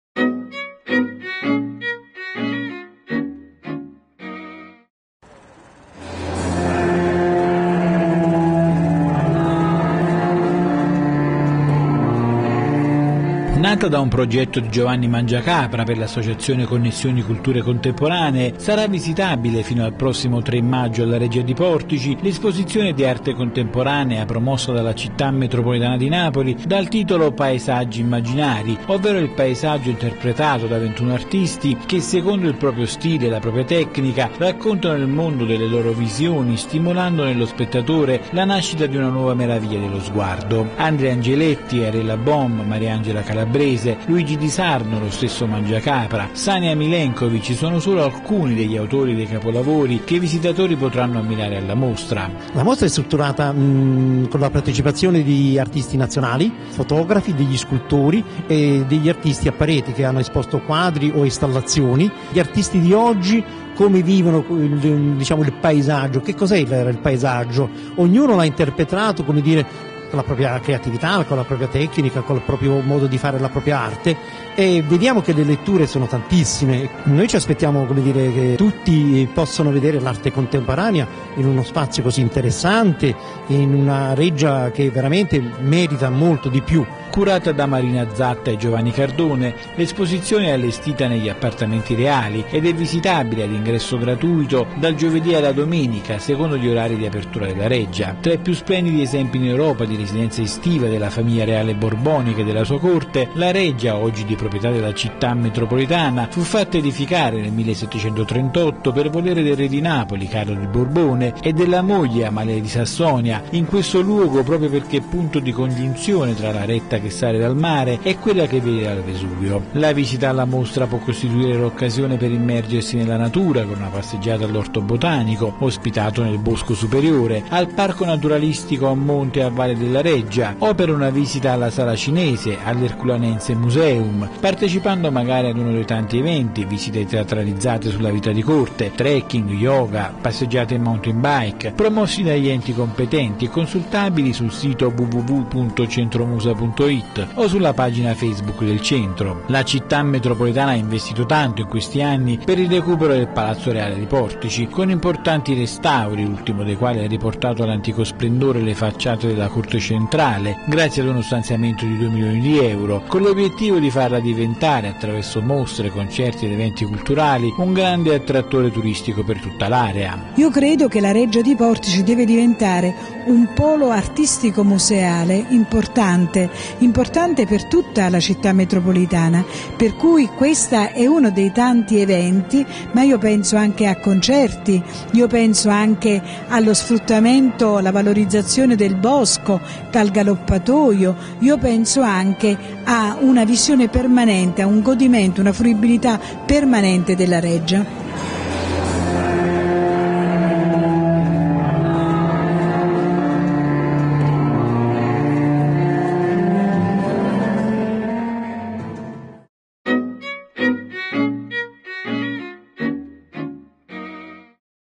“Paesaggi Immaginari”, fino al 3 maggio weekend con l’arte contemporanea alla Reggia di Portici
Dal giovedì alla domenica sarà visitabile gratuitamente la mostra d’arte contemporanea promossa dalla Città Metropolitana di Napoli dal titolo “Paesaggi Immaginari”, nata da un progetto dell’artista Giovanni Mangiacapra per l’Associazione Connessioni Culture Contemporanee e allestita negli appartamenti reali della Reggia borbonica di Portici.

L’esposizione, inaugurata dalla Consigliera Delegata al Patrimonio Culturale della Città Metropolitana, Elena Coccia, raccoglie pittori, scultori, fotografi che interpretano il paesaggio secondo il proprio stile e la propria tecnica, raccontando il mondo delle loro visioni stimolando nello spettatore la nascita di una nuova meraviglia dello sguardo.

Tra i più splendidi esempi in Europa di residenza estiva della famiglia reale borbonica e della sua corte, la Reggia, oggi di proprietà della Città Metropolitana, fu fatta edificare nel 1738 per volere del re di Napoli, Carlo di Borbone, e della moglie, Amalia di Sassonia, in questo luogo proprio perché punto di congiunzione tra la retta che sale dal mare e quella che viene dal Vesuvio.

La visita alla mostra può costituire l’occasione per immergersi nella natura con una passeggiata all’orto botanico, ospitato nel bosco superiore, al parco naturalistico a monte e a valle della Reggia o per una visita alla Sala cinese, all’Herculanense Museum, partecipando magari a uno dei tanti eventi (visite teatralizzate sulla vita di corte, trekking, yoga, passeggiate in mountain bike) promossi dagli enti competenti e consultabili sul sito o sulla pagina Facebook del Centro.

“La Città Metropolitana – ha affermato la Consigliera Coccia - ha investito tanto, in questi anni, per il recupero del Palazzo Reale di Portici, con importanti restauri, l’ultimo dei quali ha riportato all’antico splendore le facciate della corte centrale grazie a uno stanziamento di due milioni di euro, con l’obiettivo di farla diventare, attraverso mostre, concerti ed eventi culturali, un grande attrattore turistico per tutta l’area”.